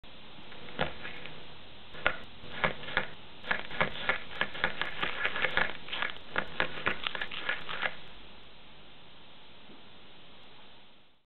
Green Onion - Animated Still Life
A stop motion animated still life of a green onion. And visit my cooking blog for recipes done artfully & with humor